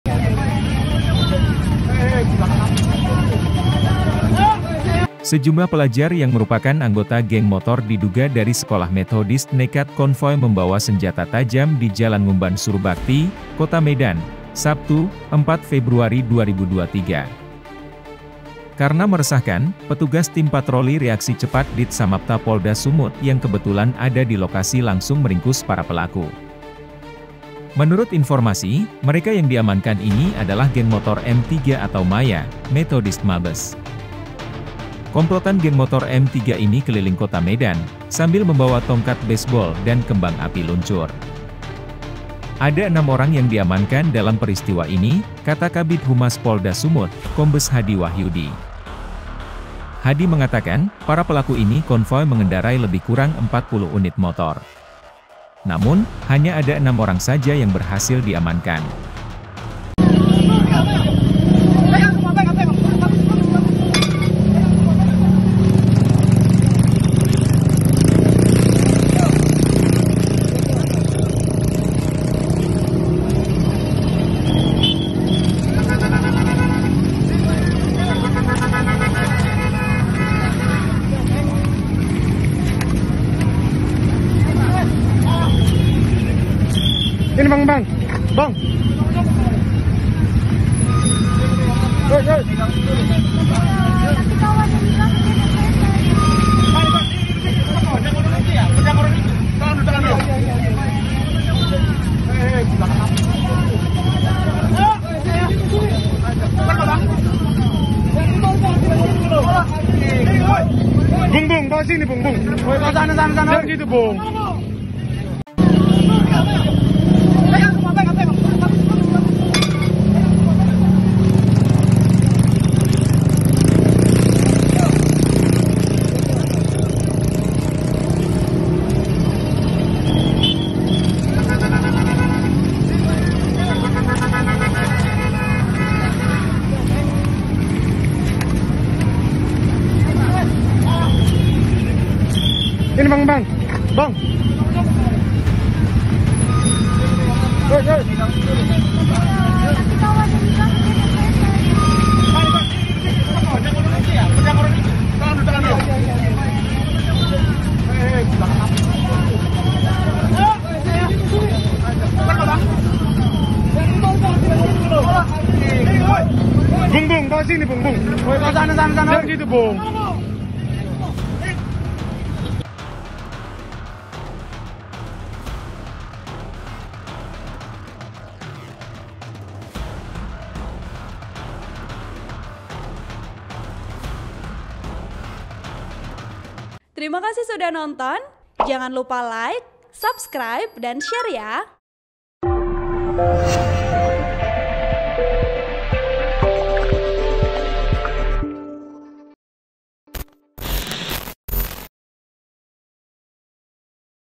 Sejumlah Pelajar Diduga Geng Motor Bawa Sajam di Kota Medan Diringkus Polisi
POS-KUPANG.COM - Sejumlah pelajar yang merupakan anggota geng motor diduga dari sekolah Methodist nekat konvoi membawa senjata tajam di Jalan Ngumban Surbakti, Kota Medan, Sabtu (4/2/2023) kemarin.

Karena meresahkan, petugas Tim Patroli Reaksi Cepat (PRC) Dit Samapta Polda Sumut yang kebetulan ada di lokasi langsung meringkus para pelaku.

Menurut informasi, mereka yang diamankan ini adalah geng motor M3 atau Maya, Methodist, Mabes.

Komplotan geng motor M3 ini keliling Kota Medan, sambil membawa tongkat baseball dan kembang api luncur.

"Ada enam orang yang diamankan dalam peristiwa ini," kata Kabid Humas Polda Sumut, Kombes Hadi Wahyudi, Sabtu sore.

Artikel ini telah tayang di Tribun-Medan.com dengan judul Geng Motor Diduga Siswa Methodist Konvoi Bawa Sajam di Kota Medan, PRC Sigap Tangkap Pelaku.

Video: ig@terang_media
Vpo: dhev